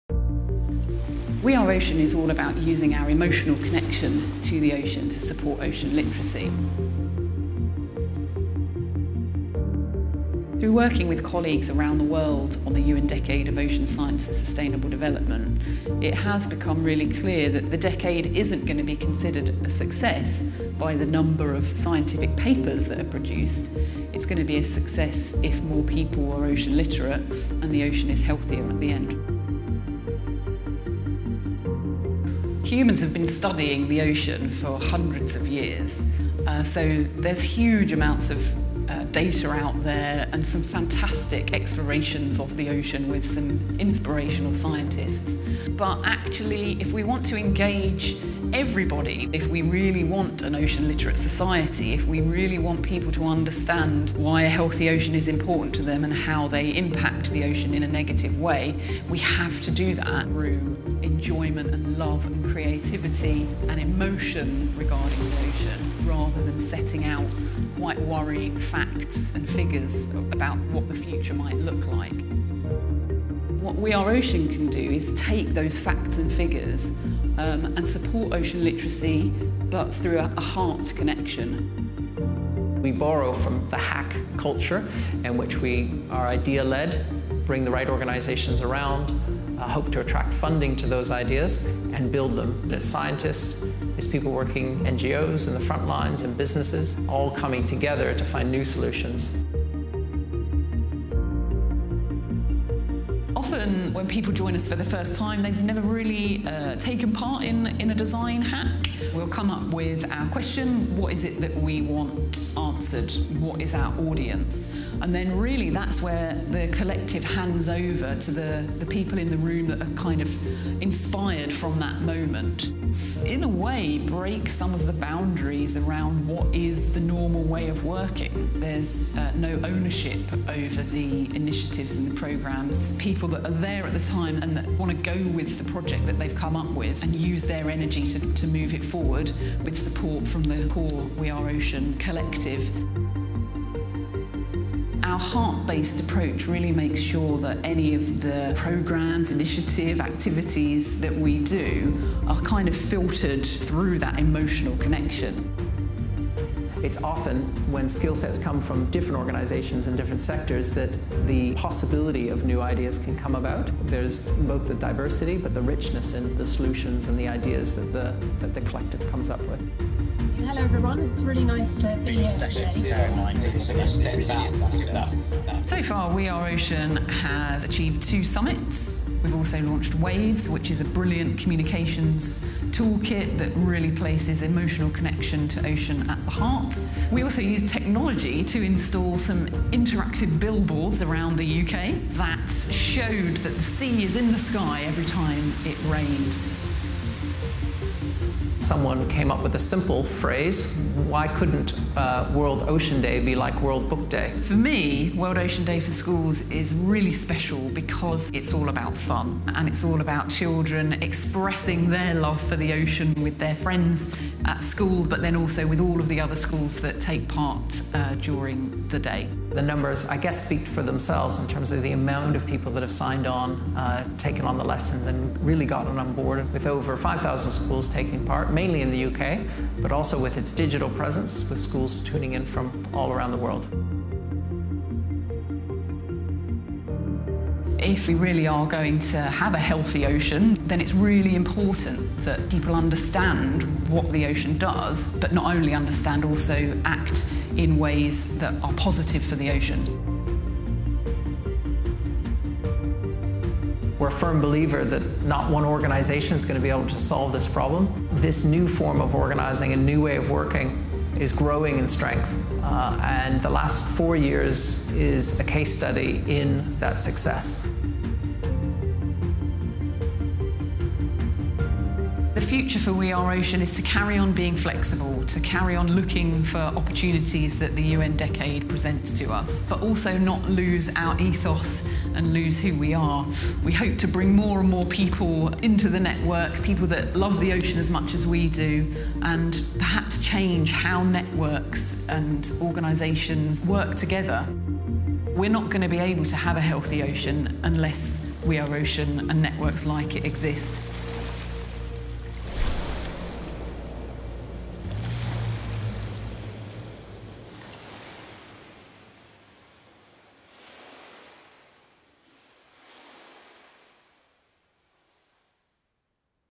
We Are Ocean campaign
We Are Ocean collective emerged in 2016 from a small group of organisations, aligned by shared objectives of increasing Ocean Literacy and effective collaboration. In accelerating Ocean Literacy we will live in a society that understands the importance of the ocean to humankind, can talk about the ocean in a meaningful way and are able to make informed and responsible decisions regarding the ocean and it’s resources.